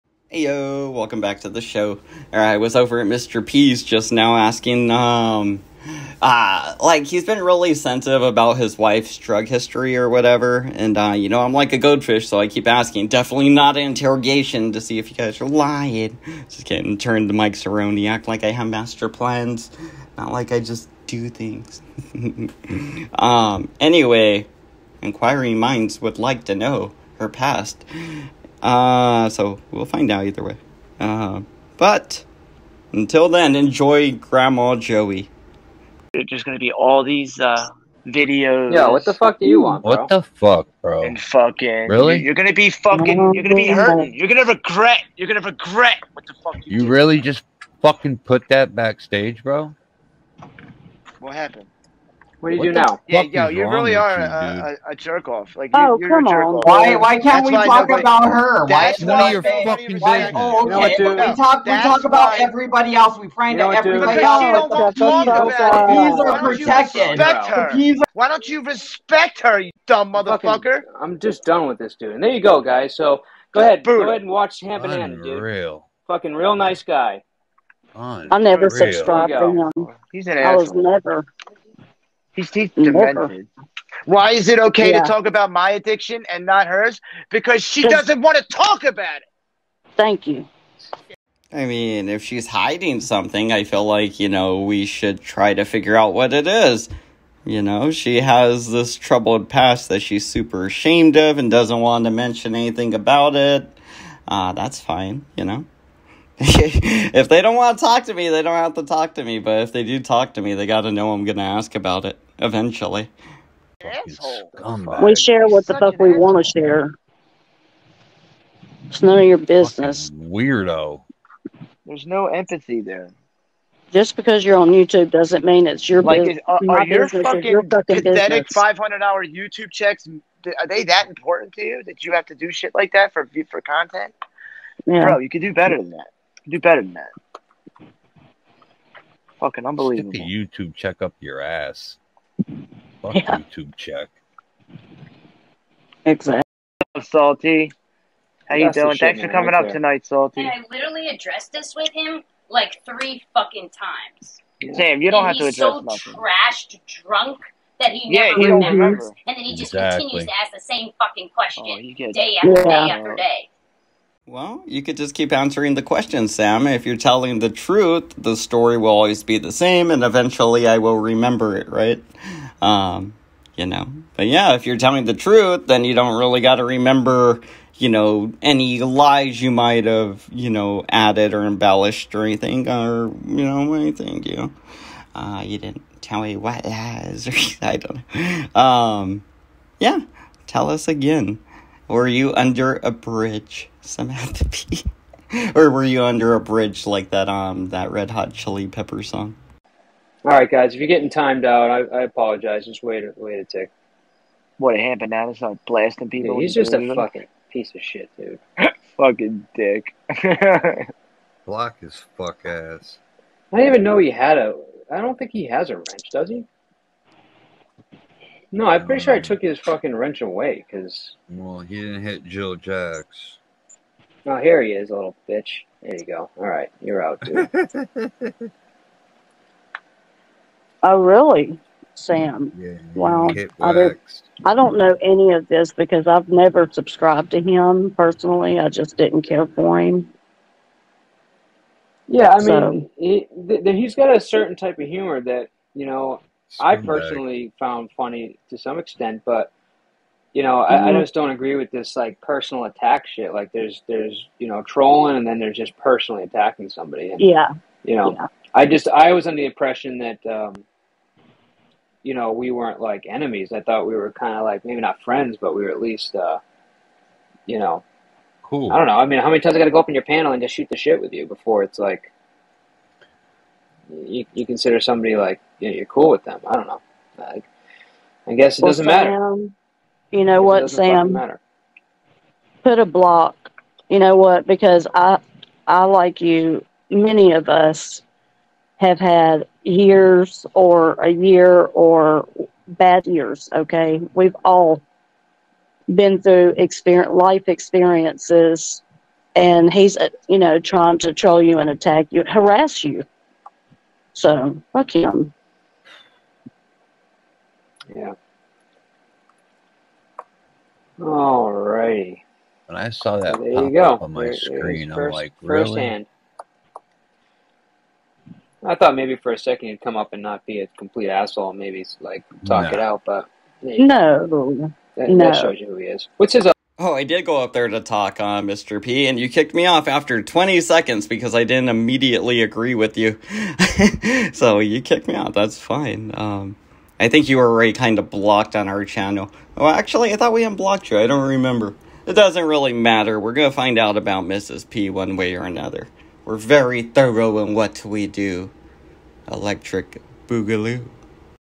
Asking Mr P About His Wife's Past Drug Abuse 🤣😅🤓
It seems to really grind me p’s gears if you ask about his wife’s past 🤔🤫🫢